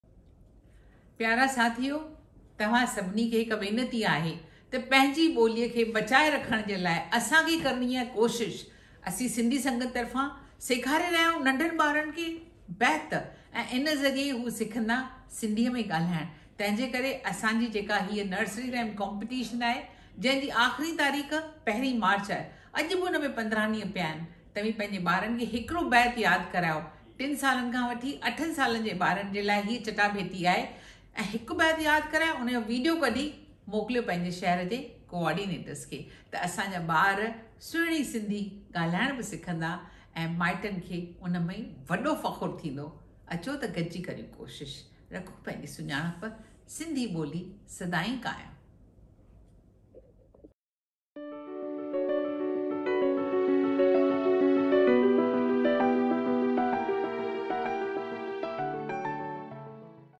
Request you to teach Sindhi poem سيکاريو سنڌي بيت
ِSindhi Nursery rhymes are extended till 1st MArch 2023.
Teachers are trquested to teach every Sindhi kid in your school who is 3 to 8 years of age group.
16 Cities are holding this competition. join in to promote Sindhi language.
Organized by Asha Chand.
We promote Sindhi through the internet and media ie Television.
to view all the content so far produced by us